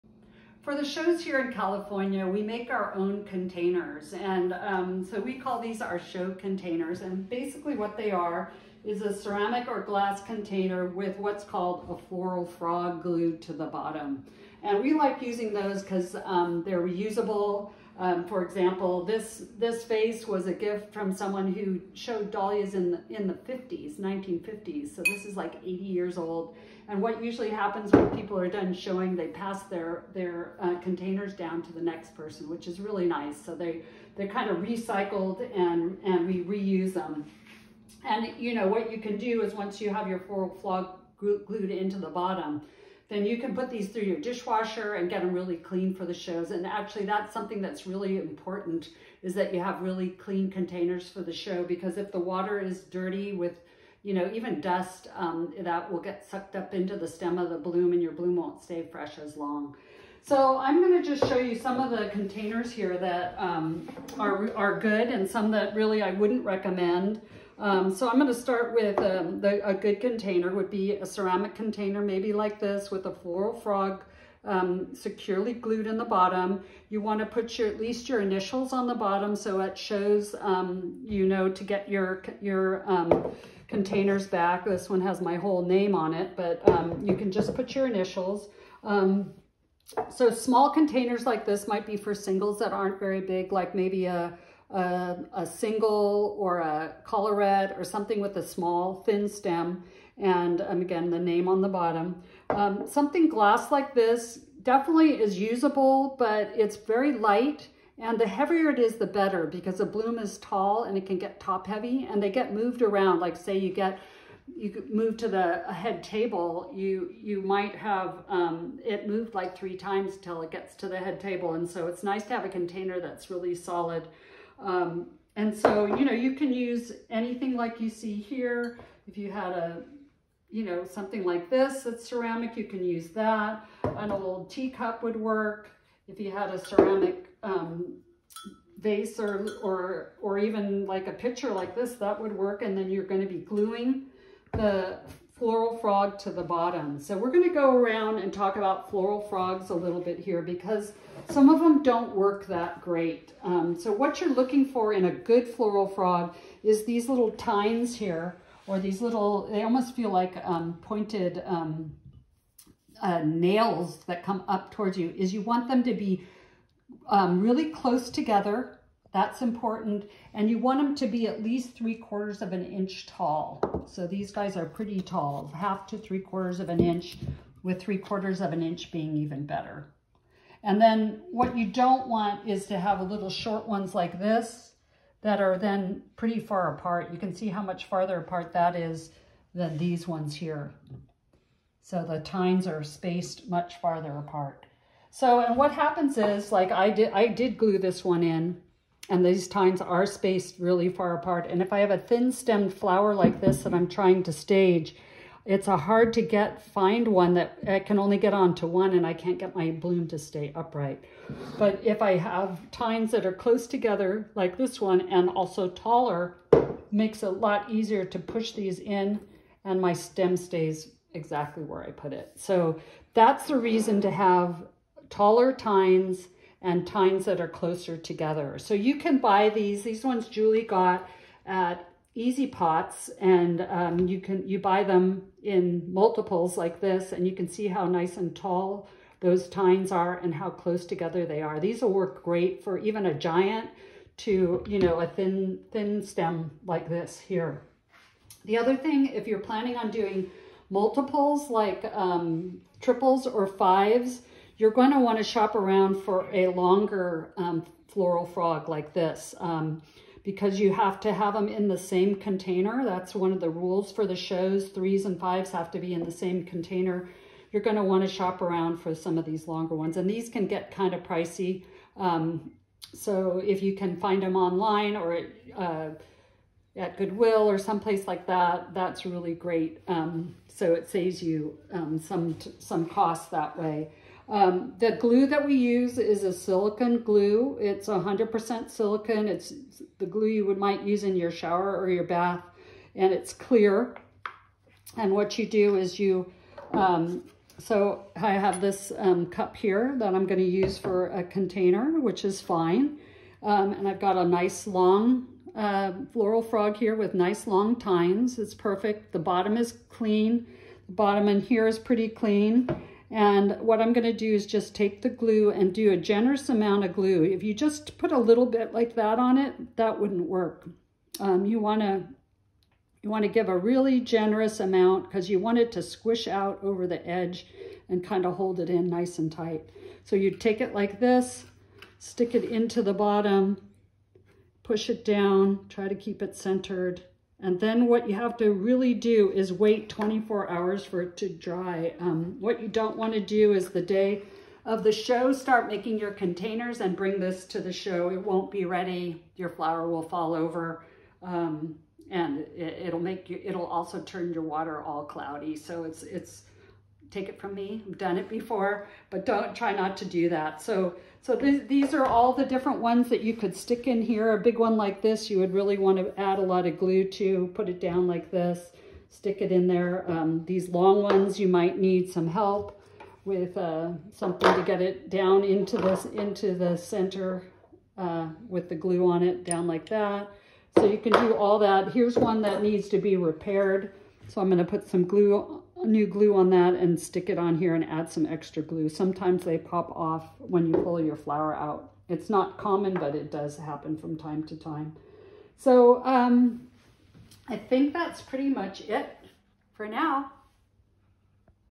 167. Using Pin Frogs in Containers to Display Blooms.
This video shows how to glue a pin frog into a second-hand container for displaying individual or grouped dahlia blooms.